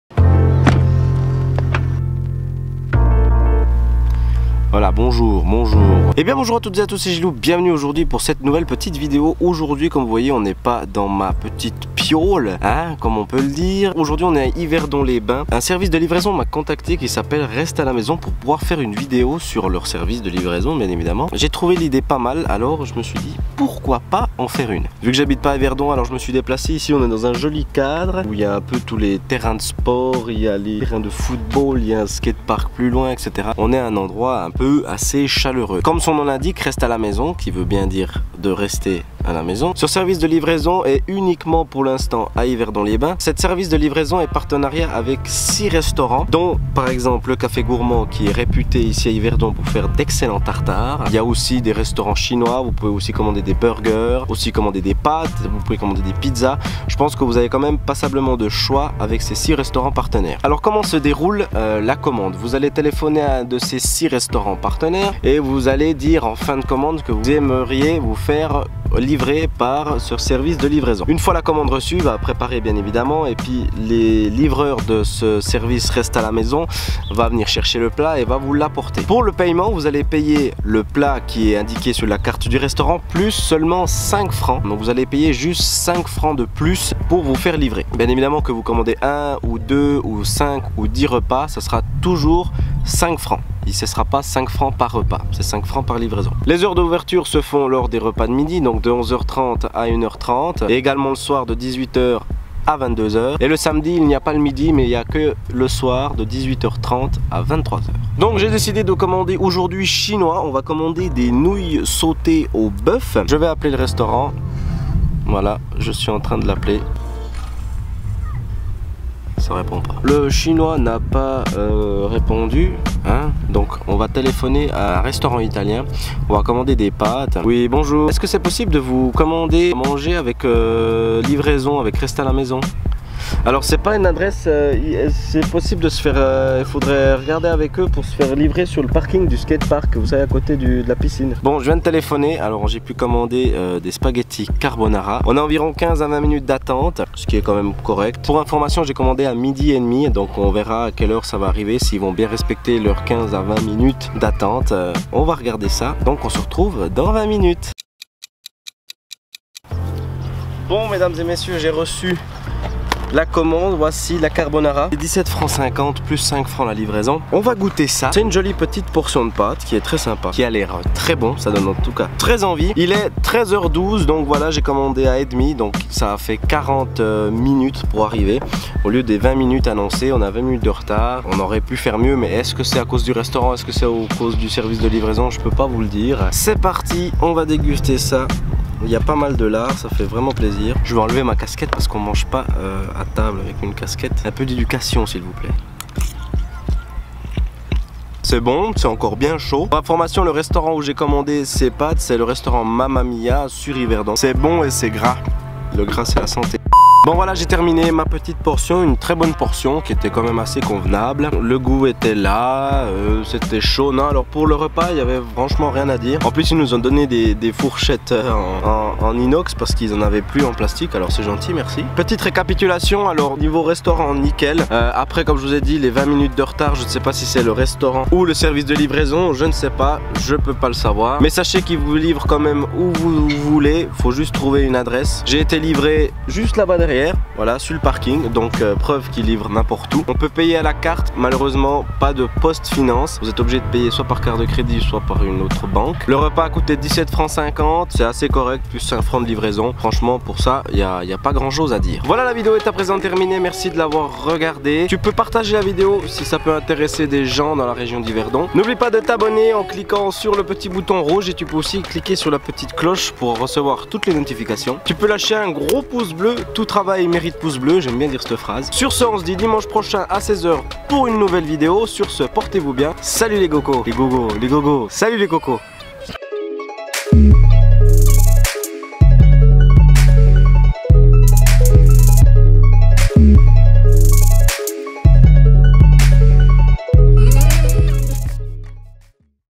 SE FAIRE LIVRER A MANGER N'IMPORTE OÙ POUR SEULEMENT 5.-
Aujourd'hui, on va tester le nouveau service de livraison "Rest'a la maison" à Yverdon-les-Bains !

--NOUVELLE VIDEO TOUS LES DIMANCHES A 16H--